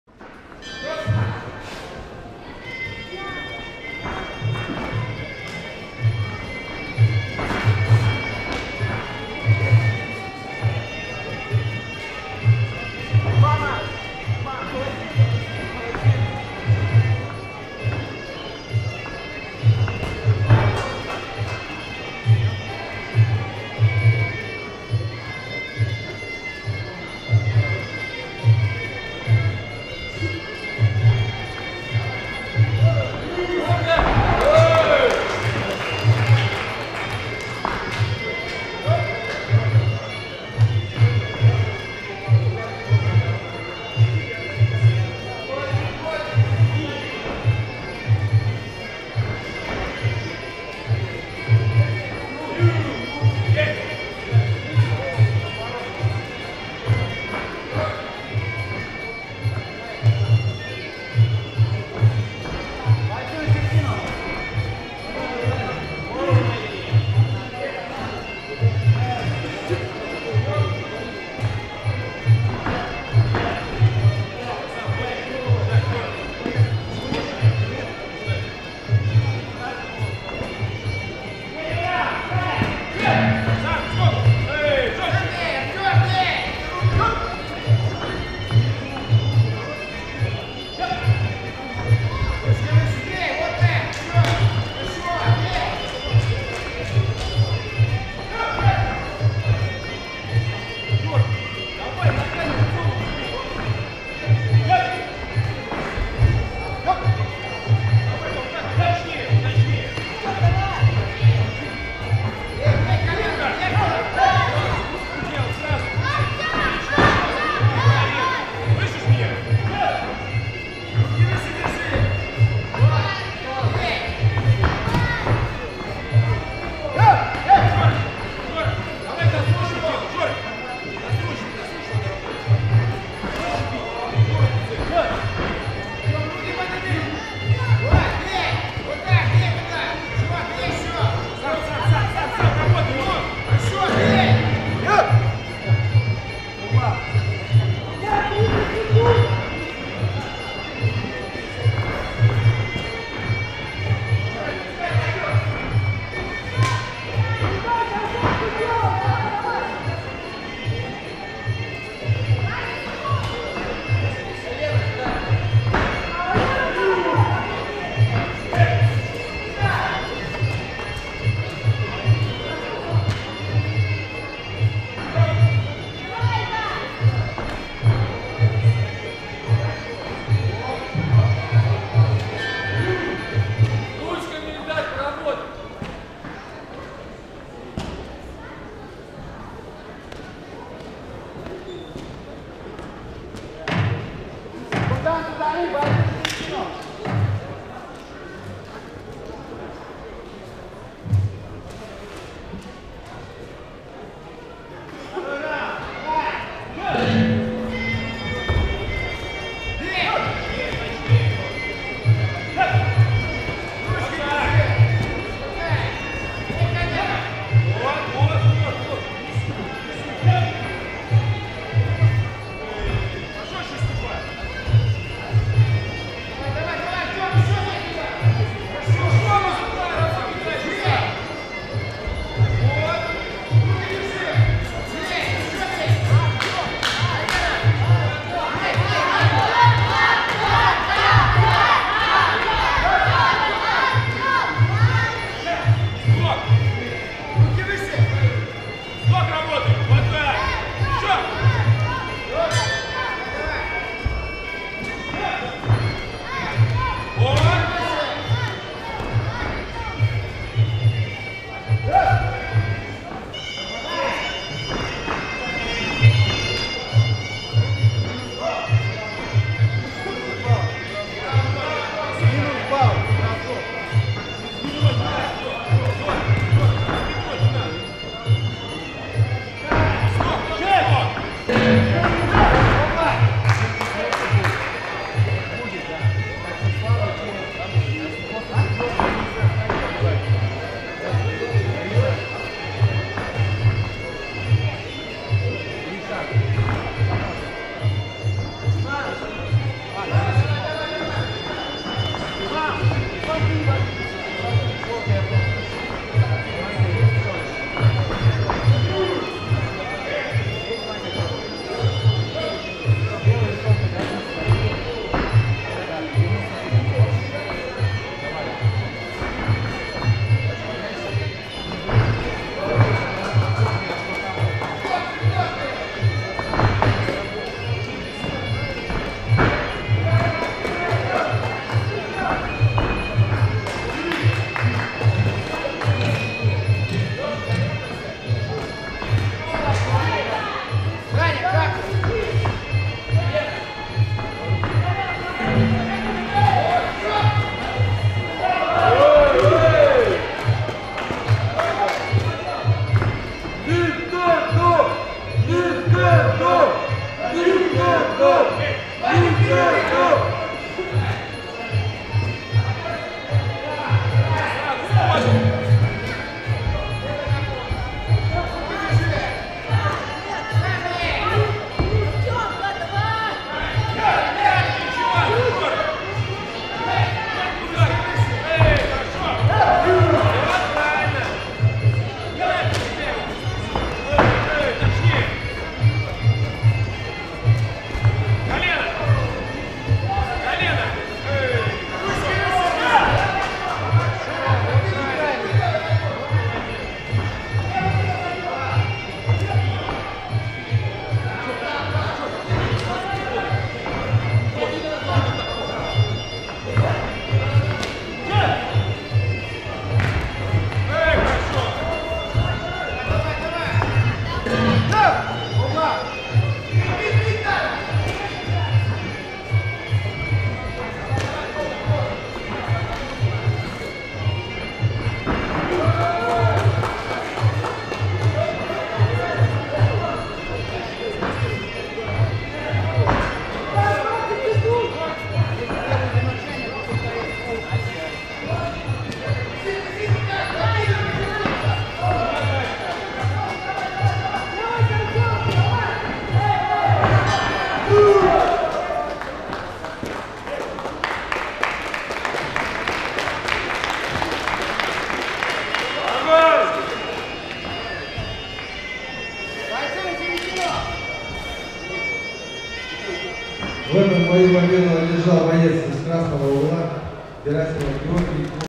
Матчевая встреча дети 30 кг Герасимов Георгий (Голден Драгон) - Саладин Артем (Боец)
11-12 февраля 2017 года
г. Санкт-Петербург
Чемпионат и Первенство Всеволожского района по тайскому